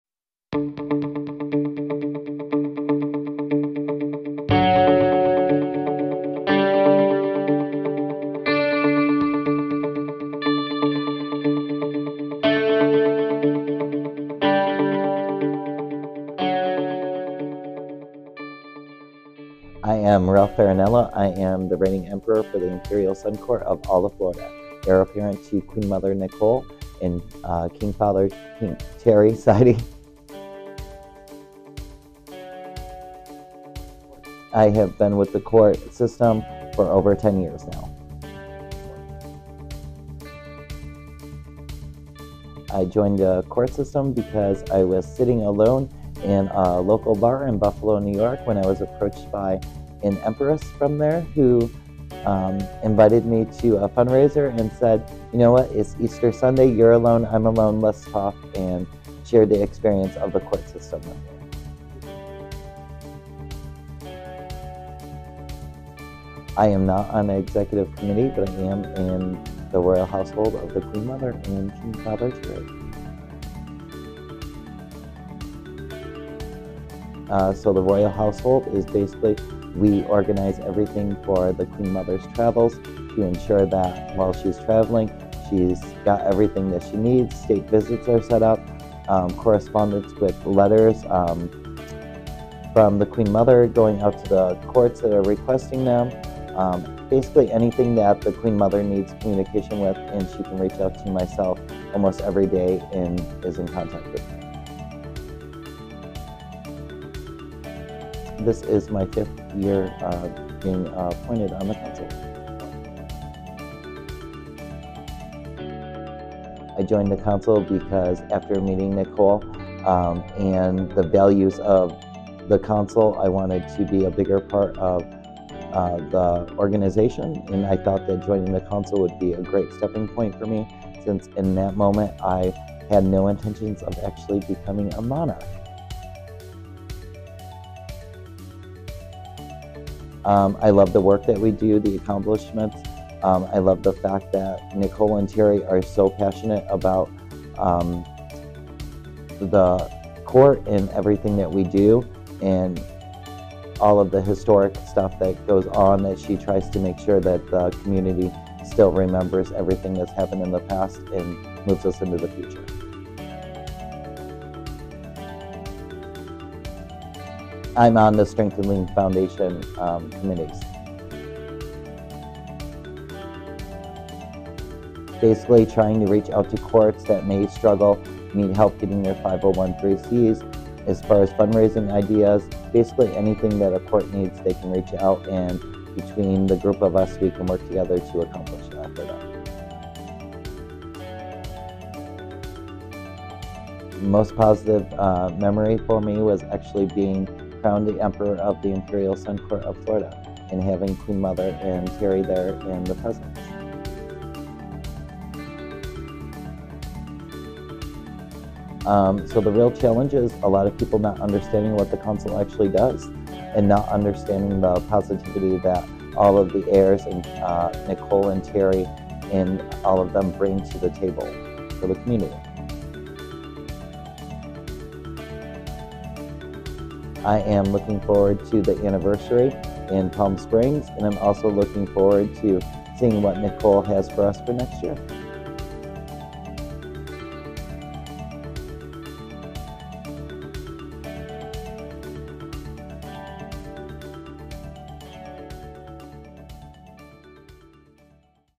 Meet The Heirs - Ralph Farinella
Meet The Heirs 2024 . . . Presenting Presenting Emperor Ralph Farinella (Florida)

I have seen Ralph on the Circuit ever since I joined the ICS world and with each meeting, I get to know him better and I enjoy him more and more.  Such a fun person with a dedication to this world.  Here is Ralph’s ICC story in 10 short questions.